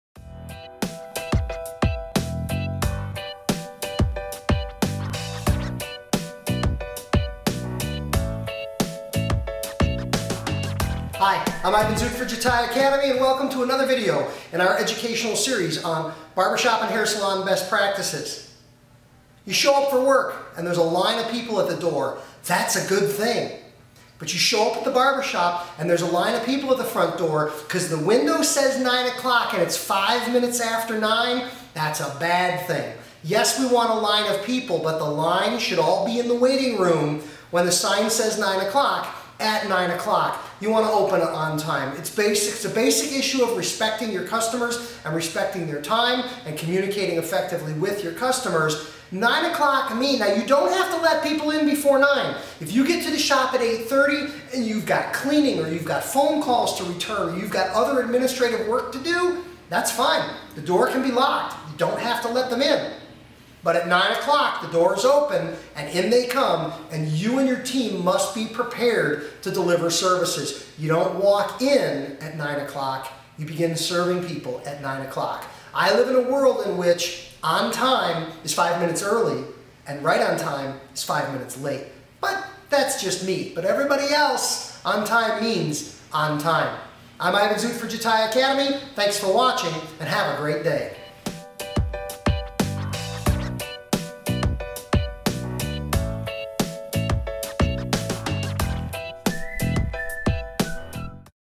Open On Time - Best Practices for Hair Professionals Series
This best practice is to open your salon or barbershop on time.